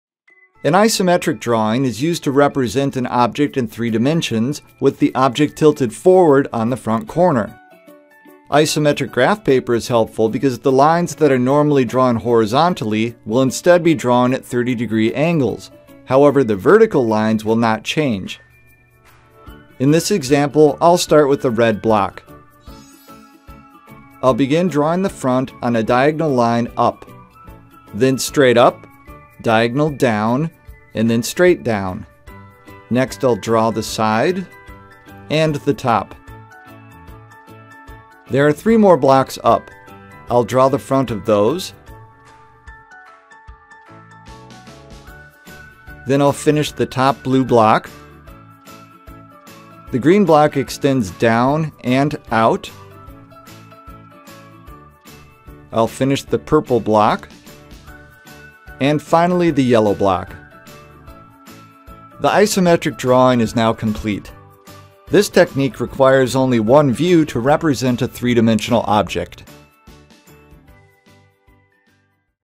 Isometric Drawing - Simplified
Learn how to make an isometric drawing with simple instructions. From the Intro to Engineering & Design curriculum by Paxton/Patterson College & Career Ready Labs.

Produced By: Peter Deuschle
Voice-over: Peter Deuschle